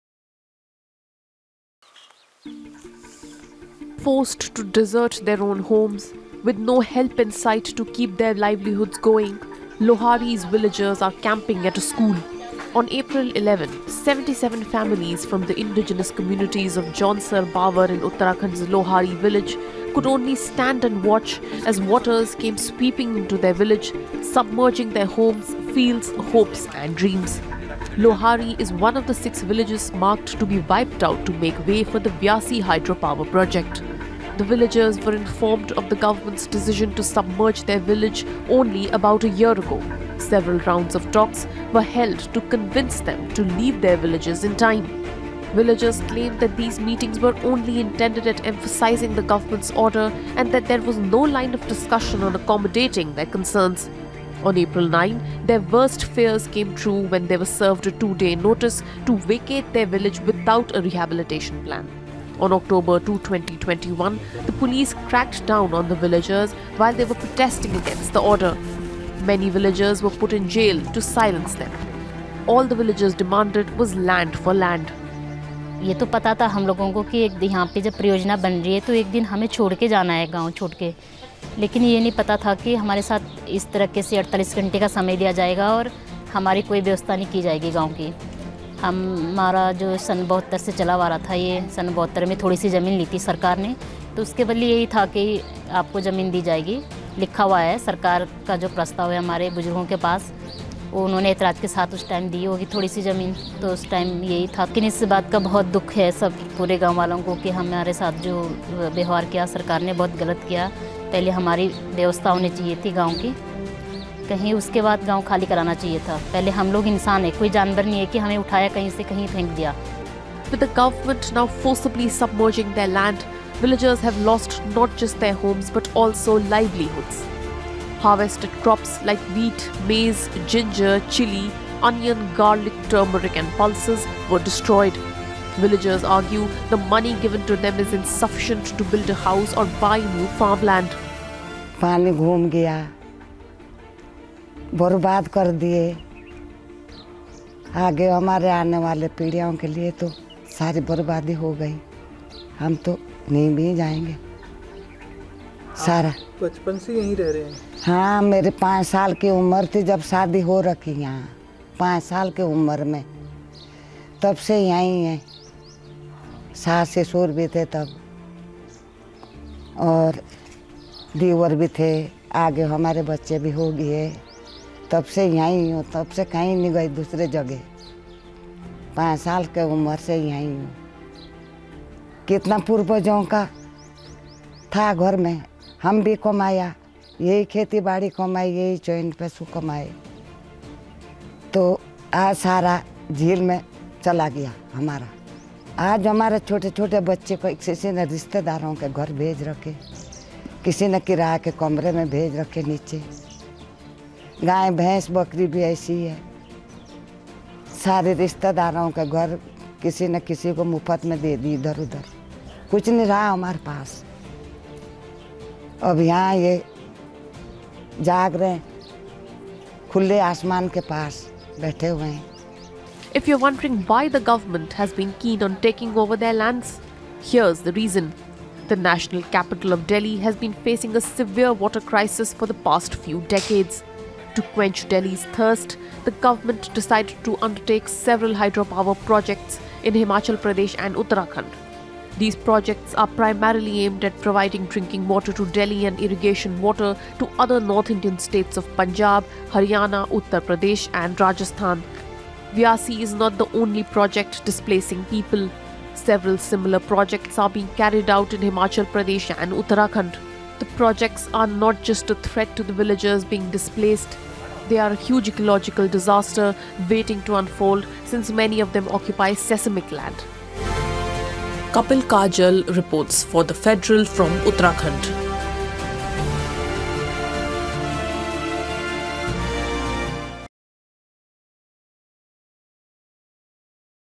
Displaced Lohari villagers left to fend for themselves | The Federal
Forced to desert their own homes, with no help in sight to keep their livelihoods going, Lohari’s villagers are camping at a school. On April 11, 77 families from the indigenous communities of Jaunsar-Bawar in Uttarakhand’s Lohari village, could only stand and watch as waters came sweeping into their village, submerging their homes, fields, hopes and dreams. Lohari is one of the six villages marked to be wiped out to make way for the Vyasi Hydropower Project.